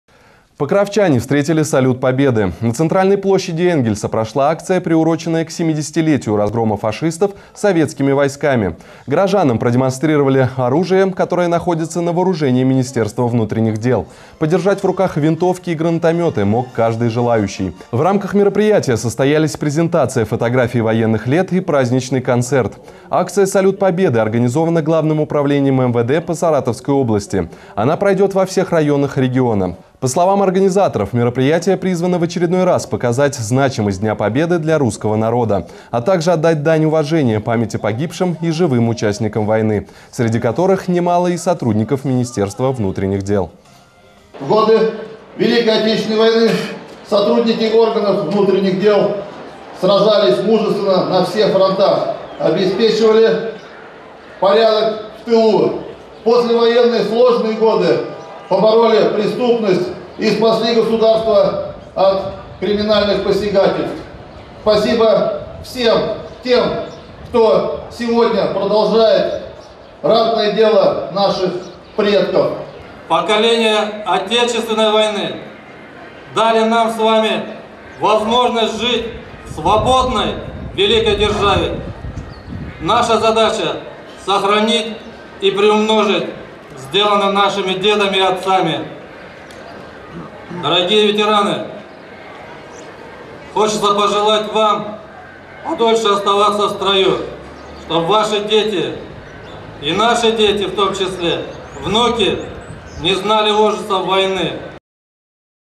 В Энгельсе прошла акция "Салют Победы"
На центральной площади Энгельса прошла акция,приуроченная к 70-летию разгрома фашистов советскими войсками. Горожанам продемонстрировали оружие, которое находится на вооружении Министерства внутренних дел. Материал вышел в эфир 28 апреля 2015 года.